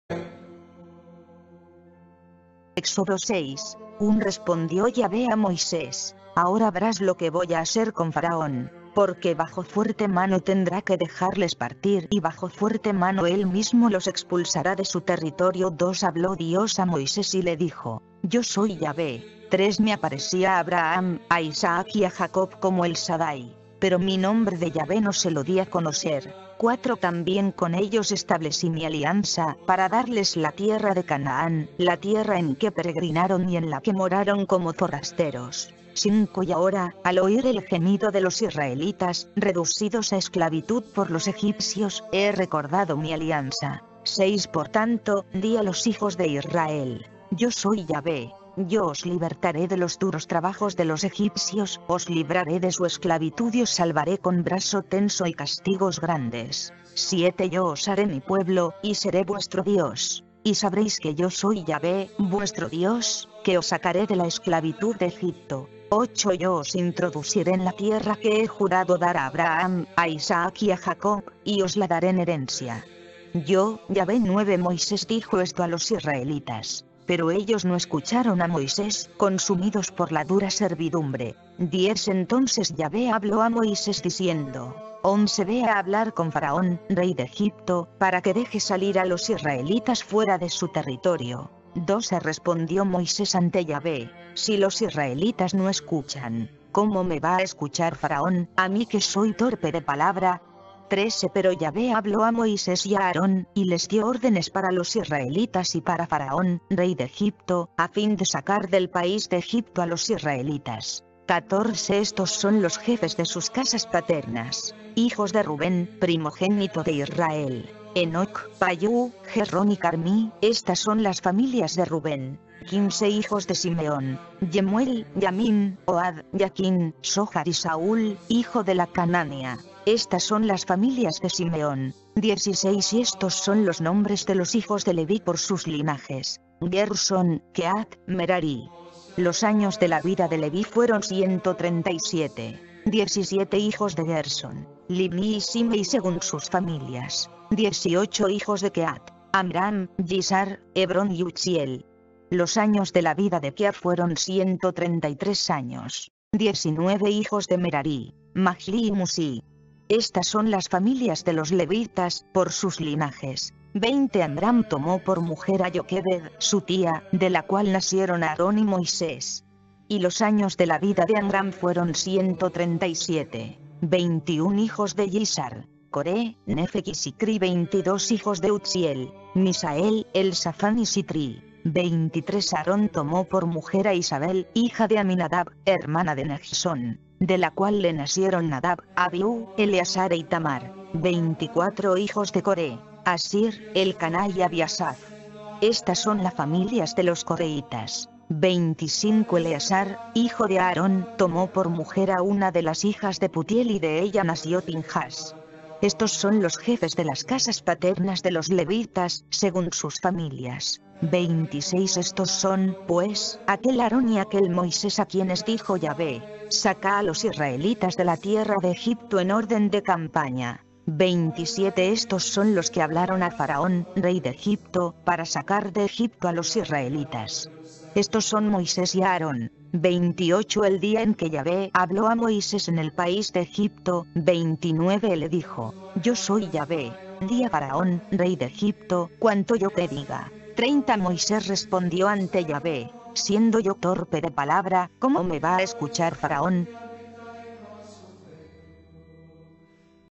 LA SANTA BIBLIA,VERSIÓN BIBLIA DE JERUSALÉN 1976, Éxodo 6
Éxodo 6
1 Respondió Yahveh a Moisés: «Ahora verás lo que voy a hacer con
Faraón; porque bajo fuerte mano tendrá que dejarles partir y bajo fuerte
mano él mismo los expulsará de su territorio.»
2 Habló Dios a Moisés y le dijo: «Yo soy Yahveh.
3 Me aparecí a Abraham, a Isaac y a Jacob como El Sadday; pero mi
nombre de Yahveh no se lo di a conocer.
4 También con ellos establecí mi alianza, para darles la tierra de
Canaán, la tierra en que peregrinaron y en la que moraron como forasteros.
5 Y ahora, al oír el gemido de los israelitas, reducidos a esclavitud por
los egipcios, he recordado mi alianza.
6 Por tanto, di a los hijos de Israel: Yo soy Yahveh; Yo os libertaré de
los duros trabajos de los egipcios, os libraré de su esclavitud y os salvaré
con brazo tenso y castigos grandes.
7 Yo os haré mi pueblo, y seré vuestro Dios; y sabréis que yo soy
Yahveh, vuestro Dios, que os sacaré de la esclavitud de Egipto.
8 Yo os introduciré en la tierra que he jurado dar a Abraham, a Isaac y
a Jacob, y os la daré en herencia. Yo, Yahveh.»
9 Moisés dijo esto a los israelitas; pero ellos no escucharon a Moisés,
consumidos por la dura servidumbre.
10 Entonces Yahveh habló a Moisés diciendo:
11 «Ve a hablar con Faraón, rey de Egipto, para que deje salir a los
israelitas fuera de su territorio.»
12 Respondió Moisés ante Yahveh: «Si los israelitas no escuchan:
¿cómo me va a escuchar Faraón, a mí que soy torpe de palabra?»
13 Pero Yahveh habló a Moisés y a Aarón, y les dio órdenes para los
israelitas y para Faraón, rey de Egipto, a fin de sacar del país de Egipto a
los israelitas.
14 Estos son los jefes de sus casas paternas: Hijos de Rubén,
primogénito de Israel: Henoc, Pallú, Jesrón y Karmí, éstas son las familias
de Rubén.
15 Hijos de Simeón: Yemuel, Yamín, Ohad, Yakín, Sójar y Saúl, hijo
de la cananea; éstas son las familias de Simeón.
16 Y éstos son los nombres de los hijos de Leví por sus linajes:
Guerson, Quehat, Merarí. Los años de la vida de Leví fueron 137.
17 Hijos de Guerson: Libní y Simei según sus familias.
18 Hijos de Quehat: Amram, Yishar, Hebrón y Uzziel. Los años de la
vida de Quehat fueron 133 años.
19 Hijos de Merarí: Majlí y Musí. Estas son las familias de los levitas,
por sus linajes.
20 Amram tomó por mujer a Yokébed, su tía, de la cual nacieron
Aarón y Moisés. Y los años de la vida de Amram fueron 137.
21 Hijos de Yishar: Coré, Néfeg y Zikrí.22 Hijos de Uzziel: Missael, Elsafán y Sitrí.
23 Aarón tomó por mujer a Isabel, hija de Amminadab, hermana de
Najsón; de la cual le nacieron Nadab, Abihú, Eleazar e Itamar.
24 Hijos de Coré: Assir, Elcaná y Abiasaf. Estas son la familias de los
coreítas.
25 Eleazar, hijo de Aarón, tomó por mujer a una de las hijas de Putiel
y de ella nació Pinjás. Estos son los jefes de las casas paternas de los
levitas, según sus familias.
26 Estos son, pues, aquel Aarón y aquel Moisés a quienes dijo
Yahveh: «Sacad a los israelitas de la tierra de Egipto en orden de
campaña.»
27 Estos son los que hablaron a Faraón, rey de Egipto, para sacar de
Egipto a los israelitas. Estos son Moisés y Aarón.
28 El día en que Yahveh habló a Moisés en el país de Egipto,
29 le dijo: «Yo soy Yahveh; di a Faraón, rey de Egipto, cuanto yo te
diga.»
30 Moisés respondió ante Yahveh: «Siendo yo torpe de palabra,
¿cómo me va a escuchar Faraón?»